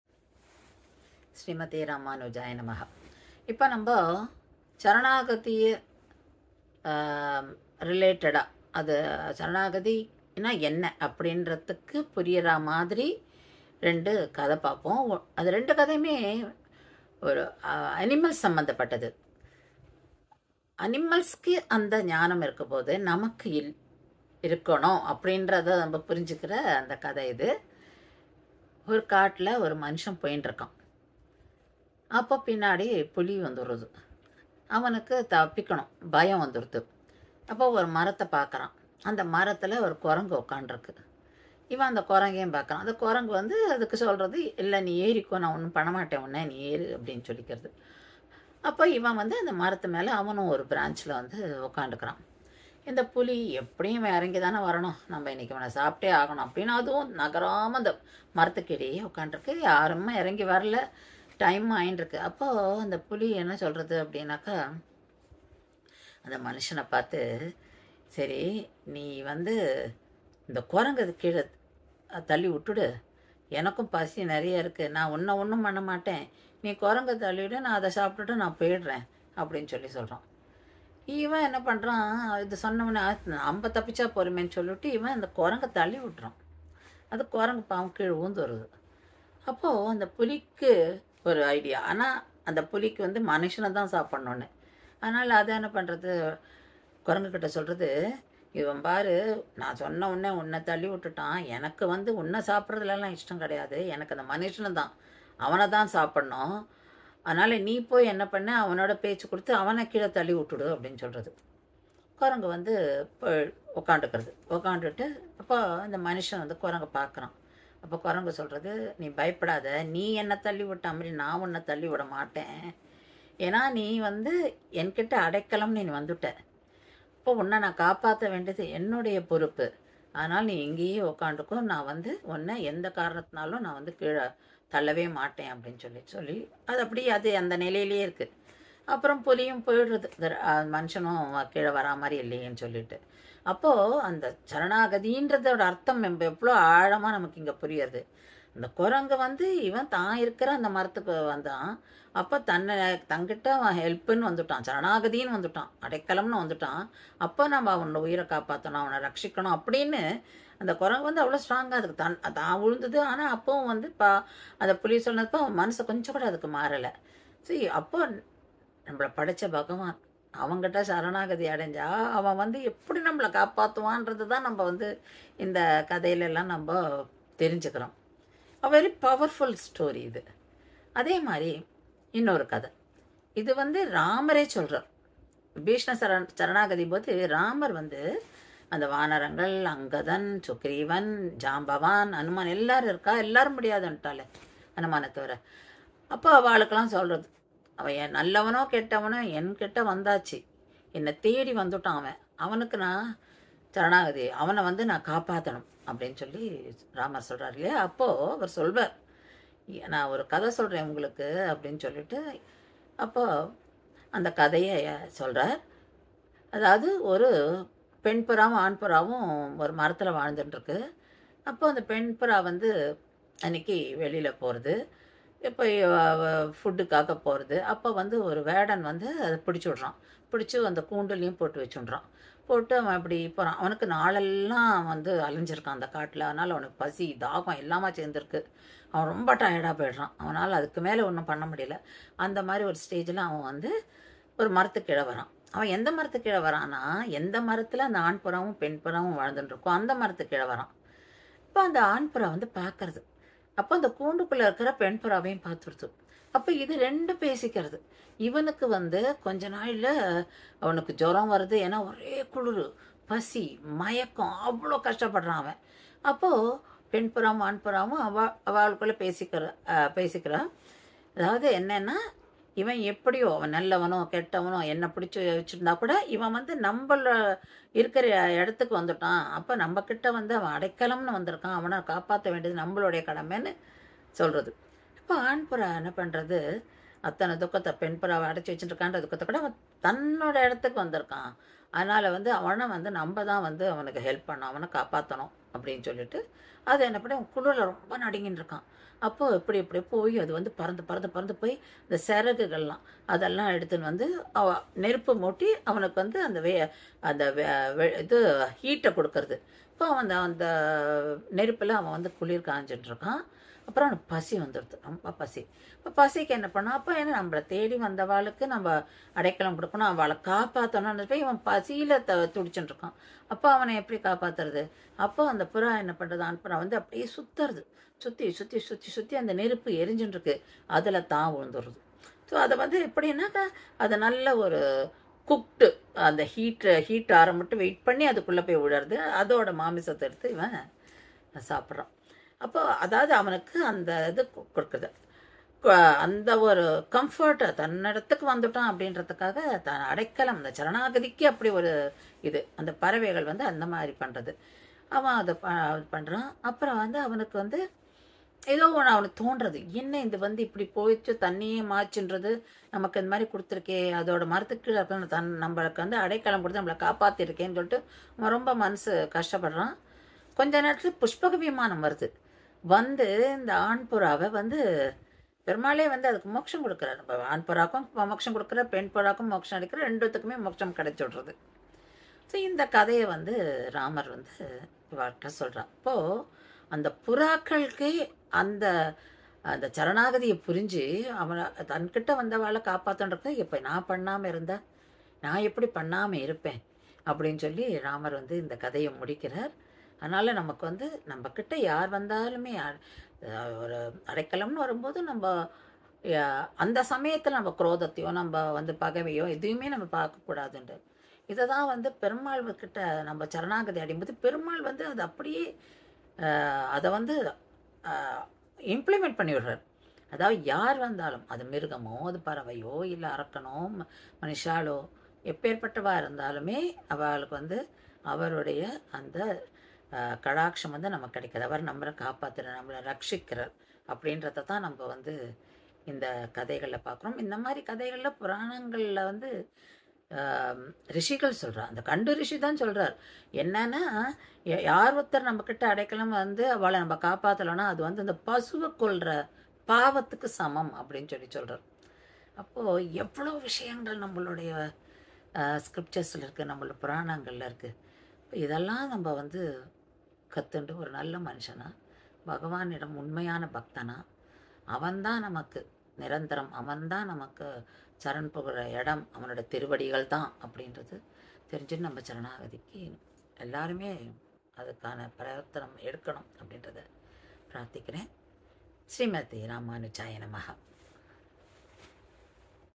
Saranagati - story narrated by Sriramar
when Vibeeshana surrendered to Sri Ramar Vanaras did not accept him. Ramar told this story to them.